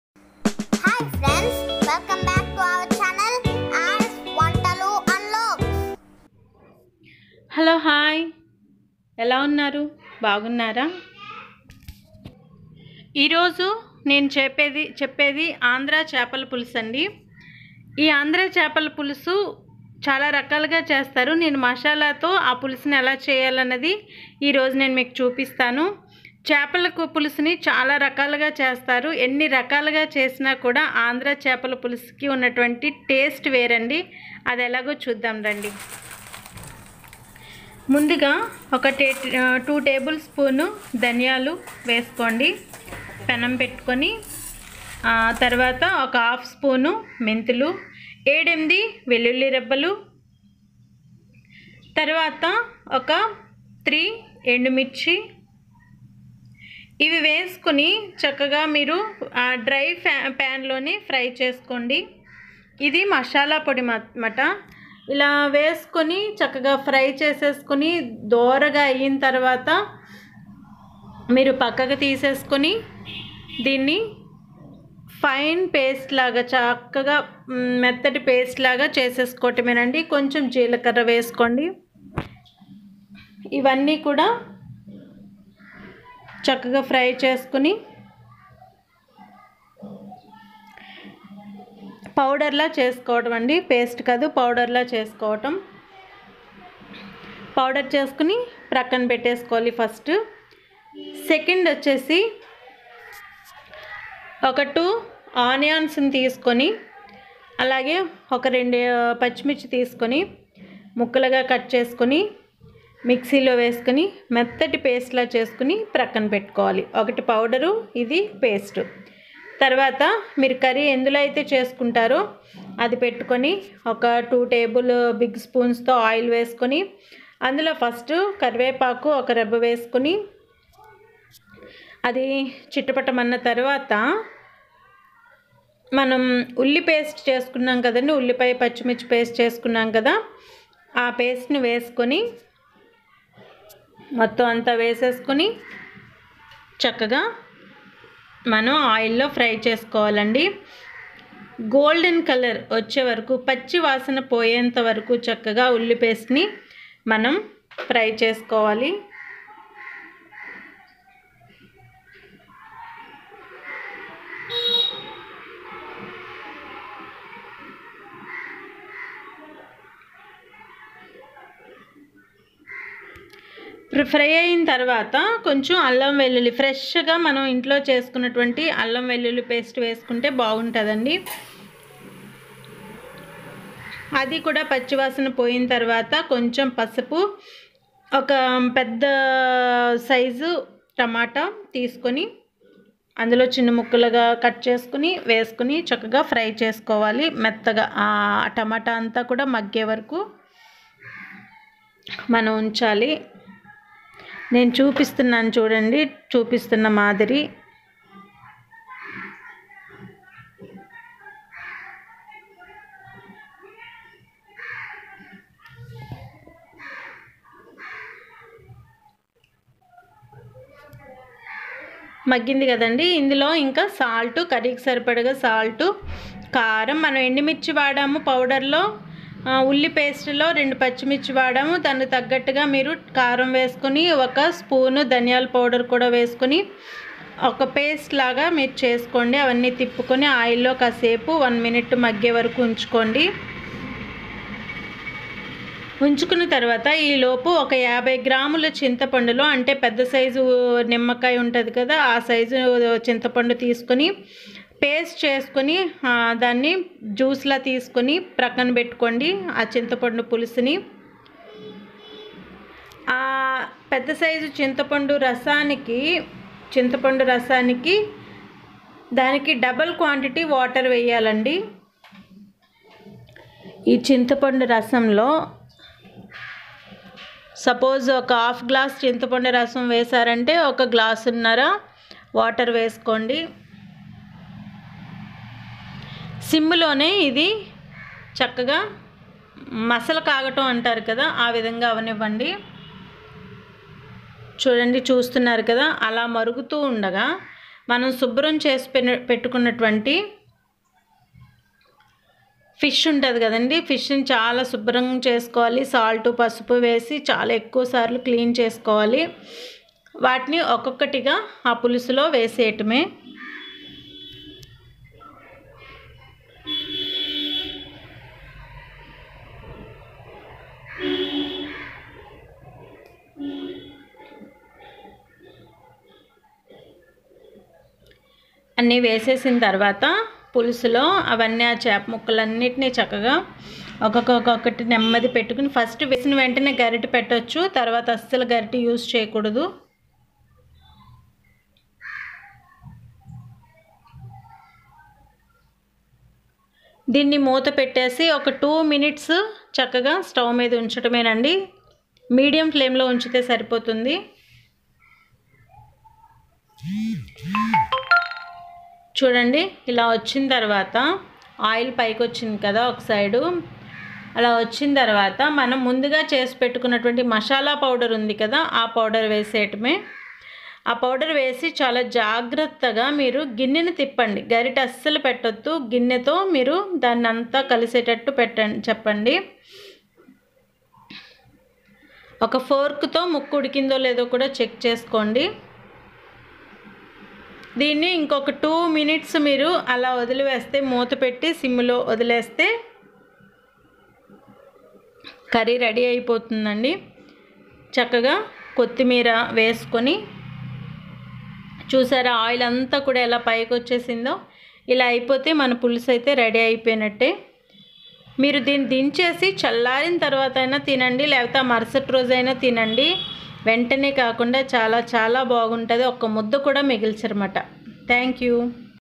మళ్ళీ మళ్ళీ తినాలనిపించే చేపల పులుసు|| Andhra Style Chepapla pulusu||Andhra style Tasty Fish Curry
This fish curry is very tasty one.You never forget about it's taste. You will eat more.